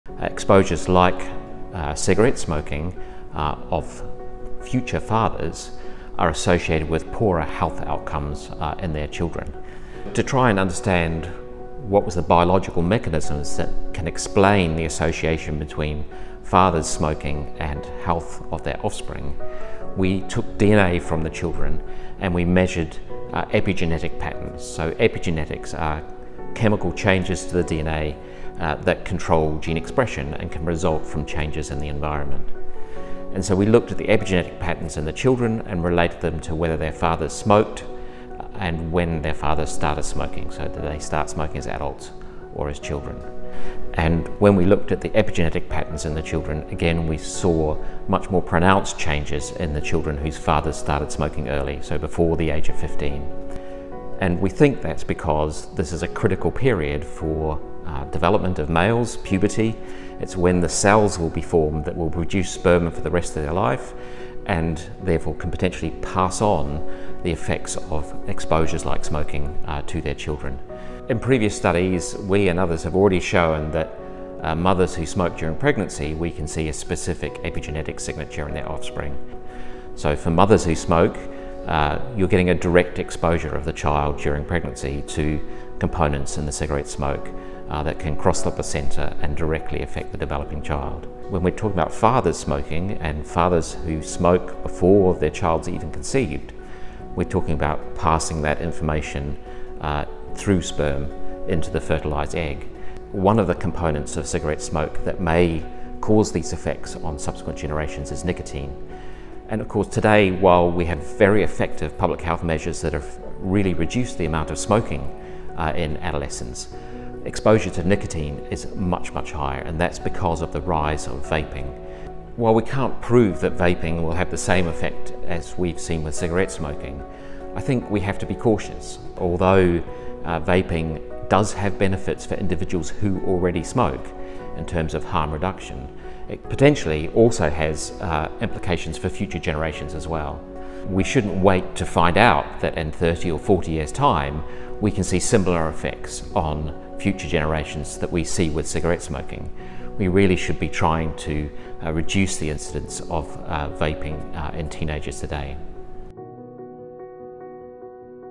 Boys smoking in early teens risk passing on harmful traits to future children
A new study suggests boys who smoke in their early teens risk damaging the genes of their future children, increasing their chances of developing asthma, obesity and low lung function.

Join us and make your mark on the world at the University of Southampton